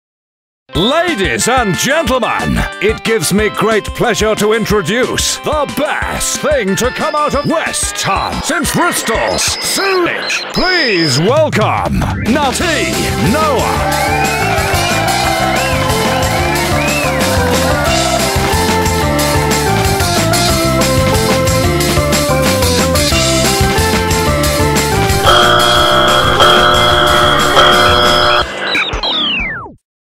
Nutty Noah Show Intro!
This is the new intro to the nutty noah show that i'm doing at the Tobacco Factory Theatre. After I get buzzed by the Britains got Talent buzzers, I stop, then the music stops and then I start the show as normal. This does need tweeking and I guess the quality will have dropped by putting it on here.